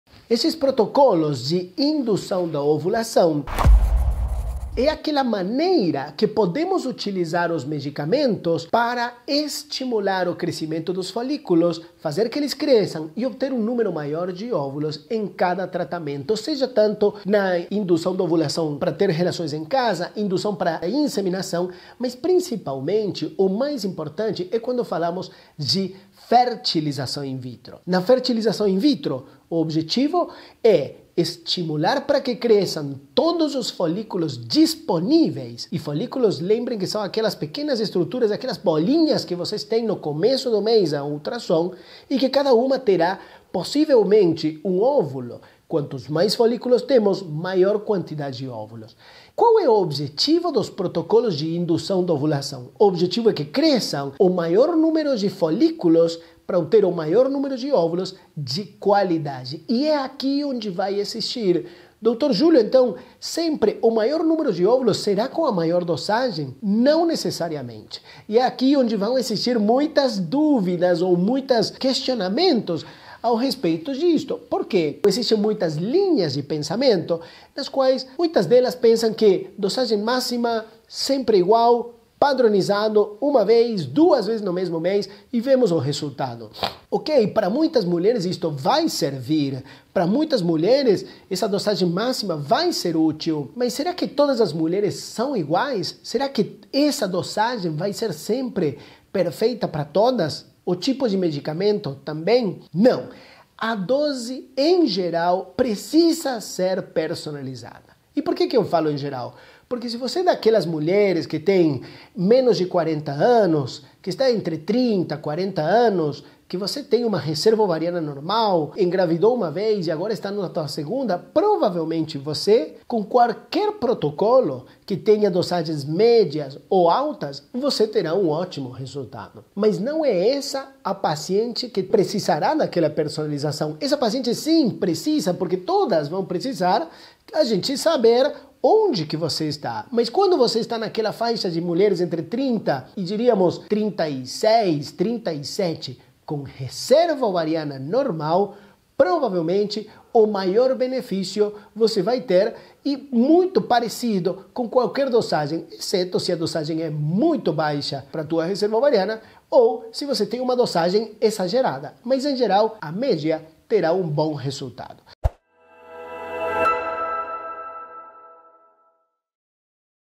QUÊ É UMA INDUÇÃO OVULATÓRIA PERSONALIZADA? | DR. JULIO VOGET
Veja quais podem ser as vantagens de uma indução ovulatória personalizada.

(8:00h às 17:00h - Segunda a Sexta-Feira)

Médico Ginecologista
CRM-SP 133.524
RQE 36.340